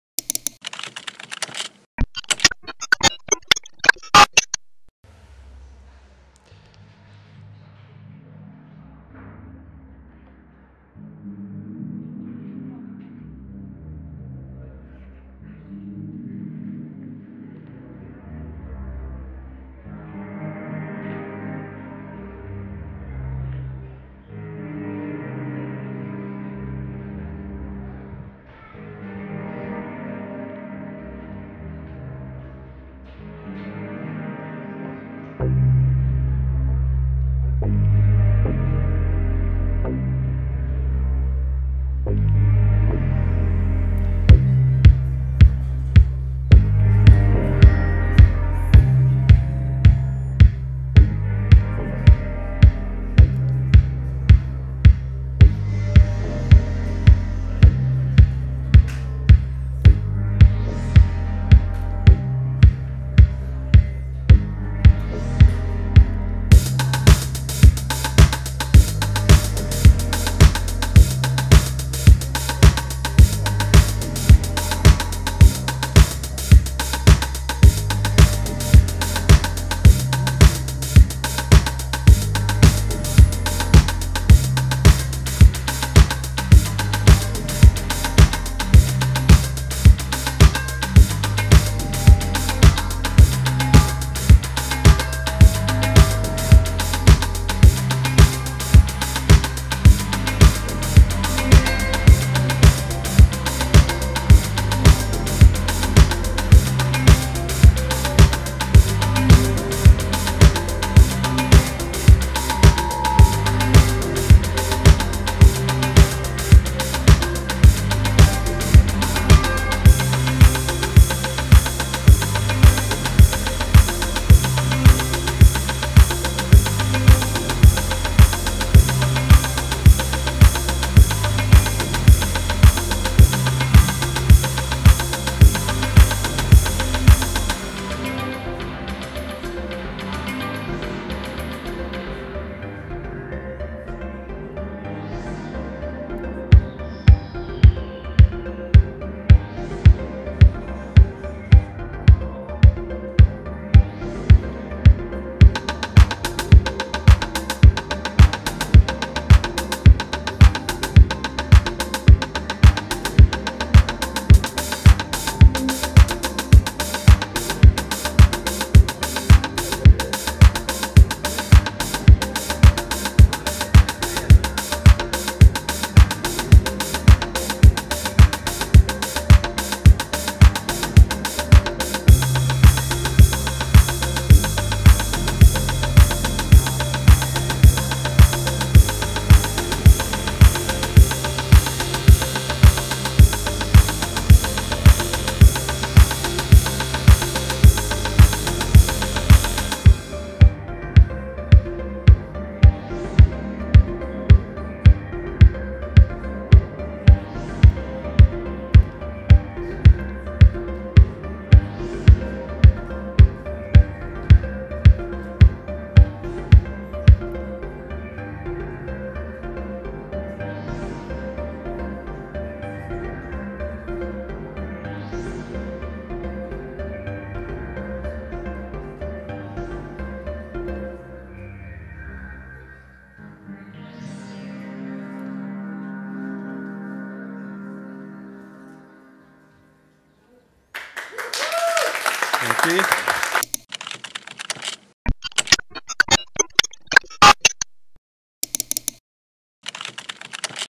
SMNEVENTS | Kuba Sojka & Novation Remote SLmk3 Workshops SMN / Synth Attack vol.4
Kuba Sojka, nauczyciel Szkoły Muzyki Nowoczesnej prezentuje rewolucyjne podejście firmy NOVATION do sterowania wieloma instrumentami MIDI za pomocą jednej klawiatury sterującej.

Jeśli twoją pasją jest tworzenie muzyki zapraszamy do Szkoły Muzyki Nowoczesnej.

Więcej na - s-m-n.pl.